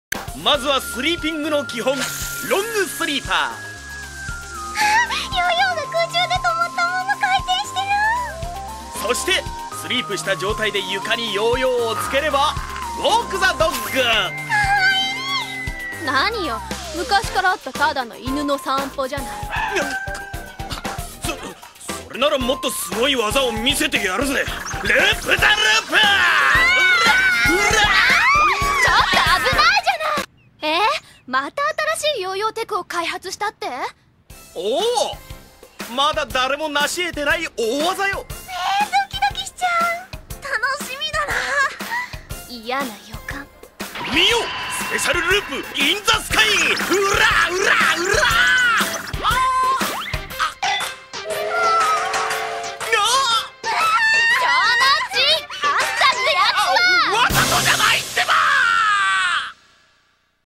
Jounouchi's Yo-Yo Tech
Jounouchi shows off his yo-yo techniques.